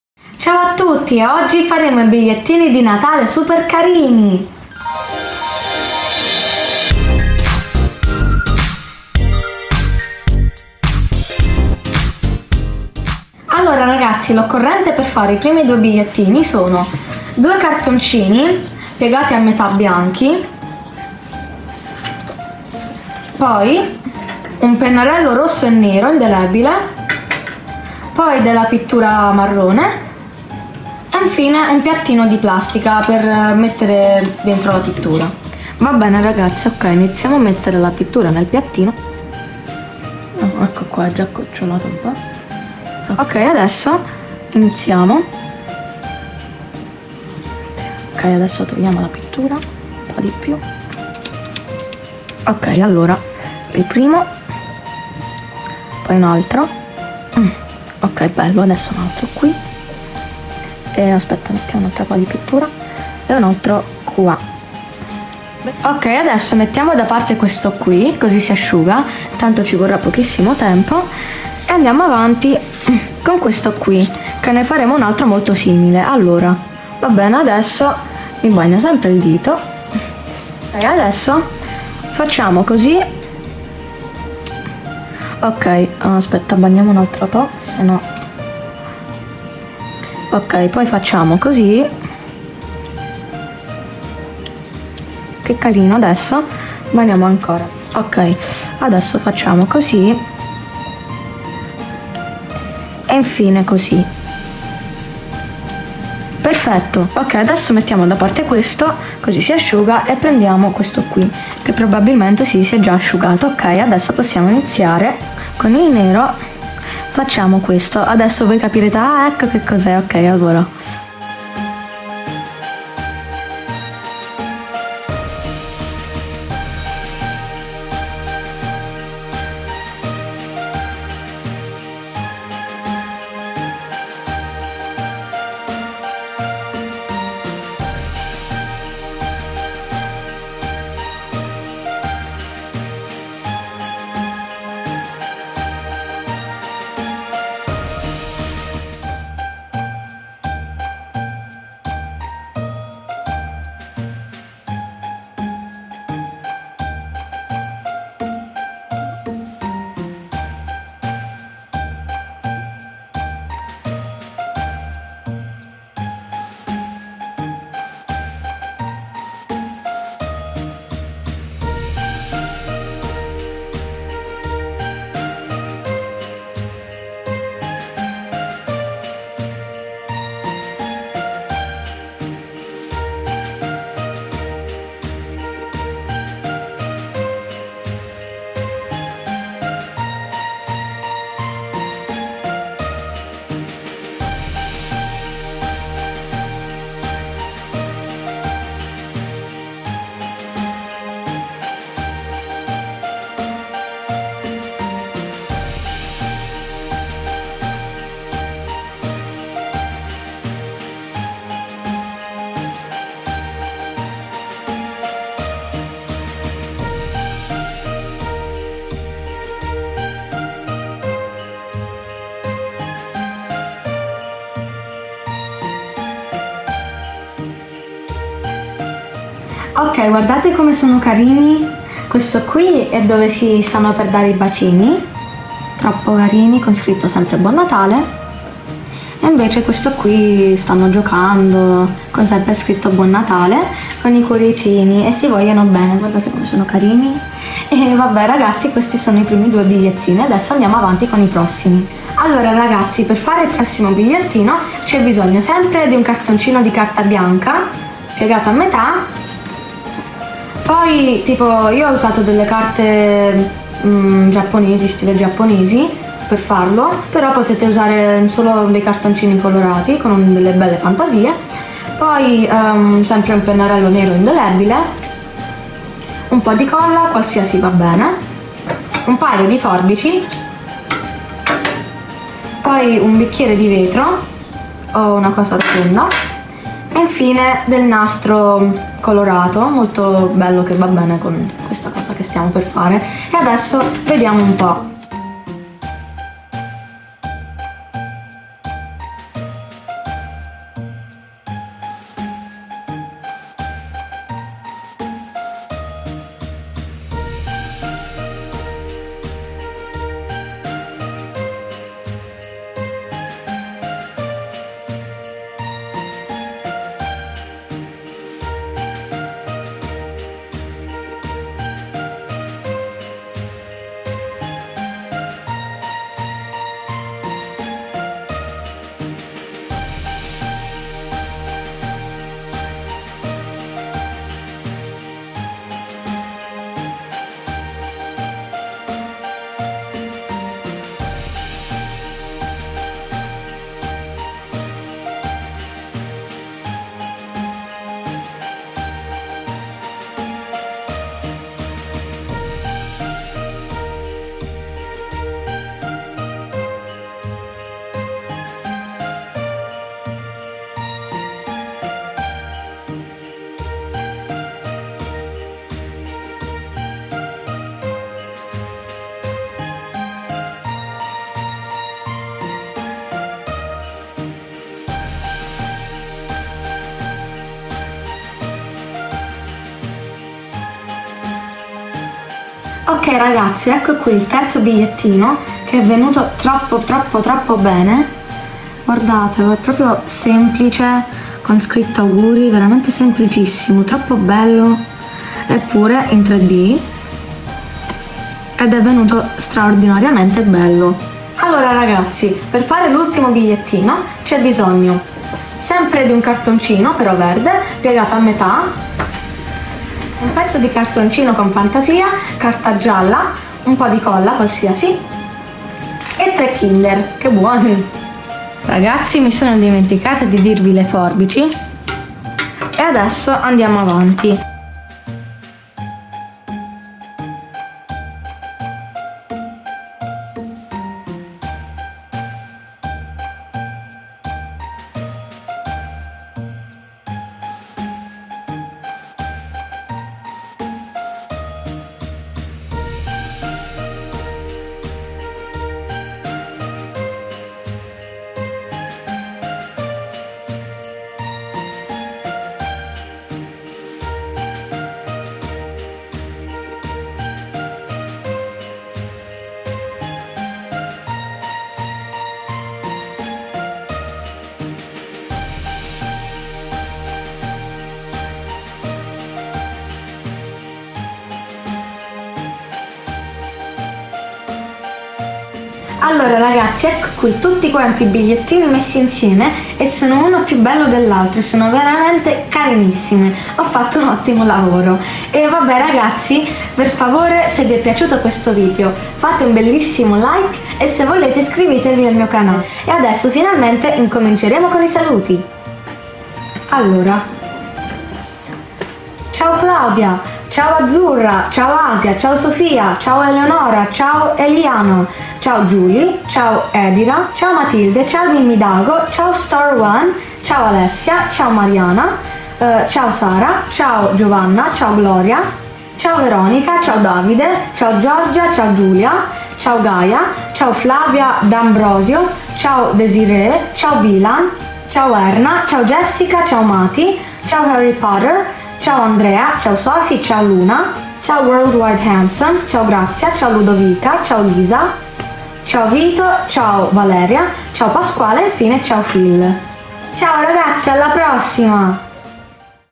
BIGLIETTINI DI NATALE- FAI DA TE! / diy Christmas Cards || SSnS
Facciamo insieme i bigliettini di Natale, super semplici e carinissimi!